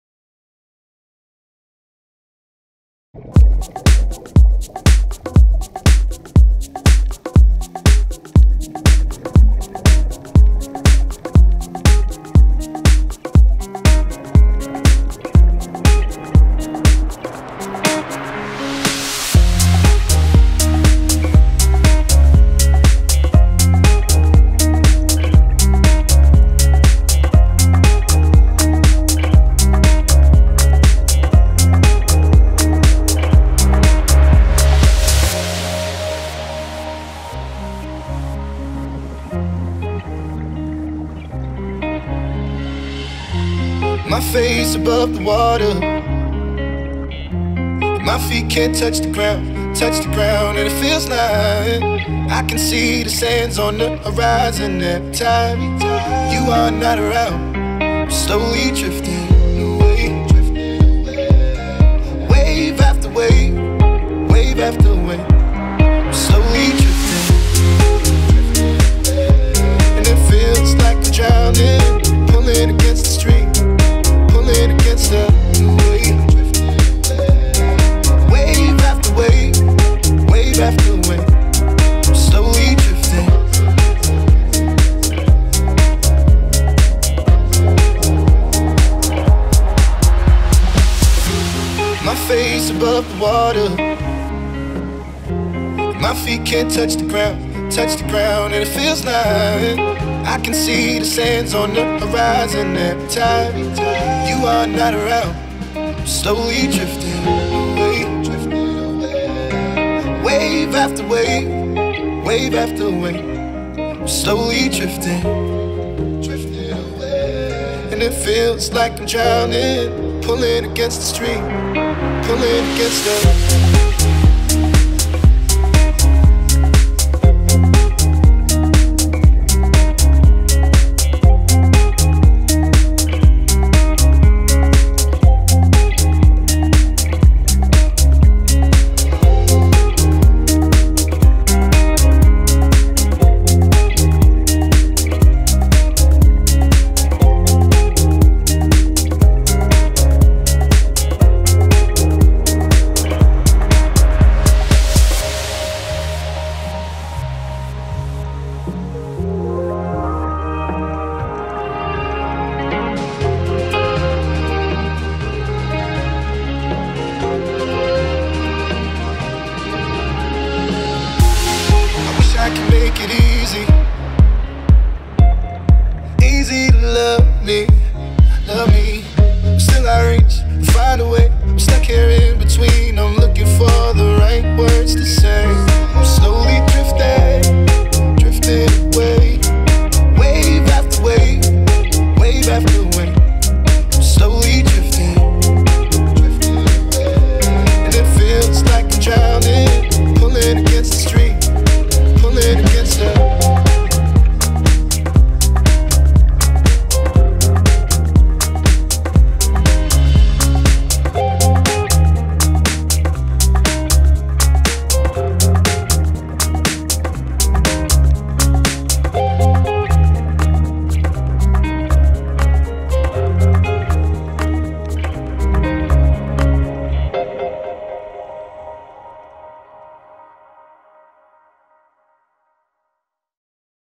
Scuba Diving in Tioman (Malaysia): Pirate Reef & Renggis
Enjoy sharks, barracudas, turtles, puffet fishes, giant morays & more :)